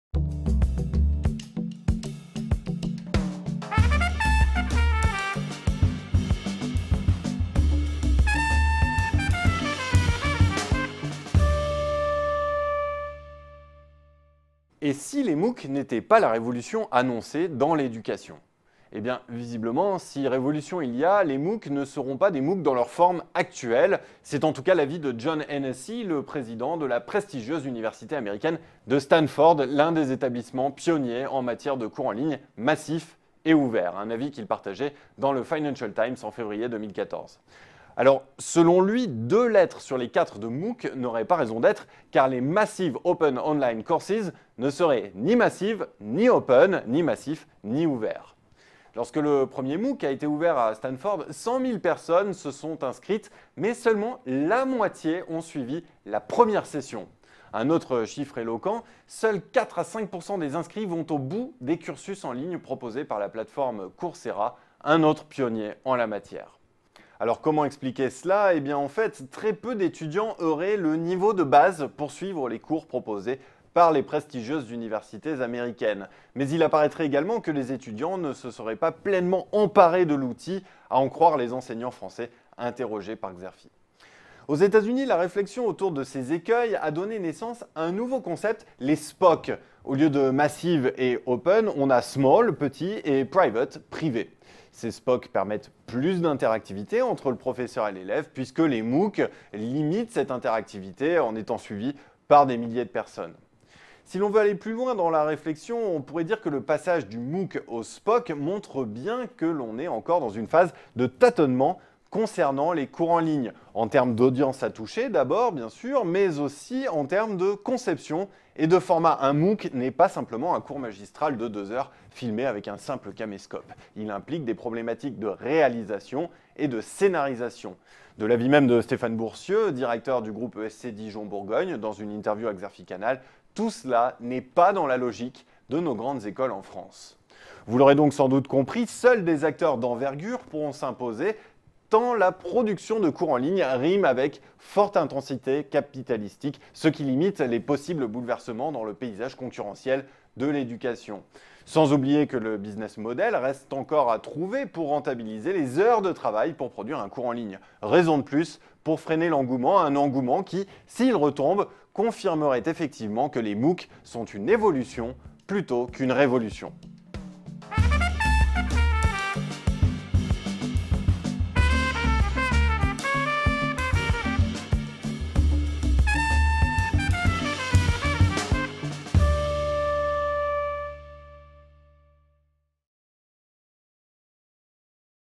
Thibault Lieurade, Xerfi Canal Les MOOC se cherchent encore
Xerfi Canal présente l'analyse de Thibault Lieurade, journaliste Xerfi Canal

Et si les MOOC n'étaient pas la révolution annoncée ? Visiblement, ce ne sera pas sous leurs formes actuelles. C'est en tous cas l'avis de John Hennessy, le président de la prestigieuse université américaine de Stanford, l'un des établissements pionniers en matière de cours en ligne massif et ouvert. Un avis qu'il partageait dans le Financial Times en février 2014.

Selon lui, deux lettres sur les quatre de MOOC n'auraient pas raison d'être, car les Massive Open Online Course ne seraient ni Massive ni Open (ni massifs ni ouverts). Lorsque le premier MOOC a été ouvert, 100 000 personnes se sont inscrites mais seulement la moitié ont suivi la première session. Autre chiffre éloquent, seuls 4 à 5% des inscrits vont au bout des cursus en ligne proposés sur la plateforme Coursera, autre pionnier en la matière. [...]

Thibault Lieurade, Les MOOC se cherchent encore, une vidéo Xerfi Canal